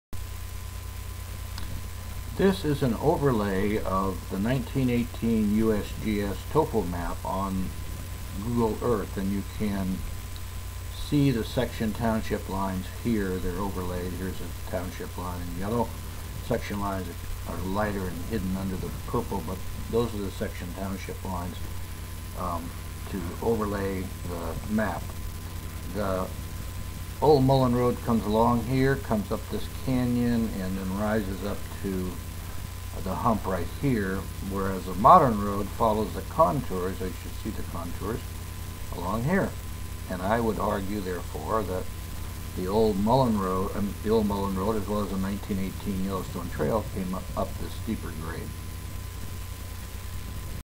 1918 Yellowstone Trail over the Hump, St regis, MT
This video shows the relationship of the 1918 mullen Road / yellowstone Trail over The Hump west of St regis, Montana to the later road over The Hump.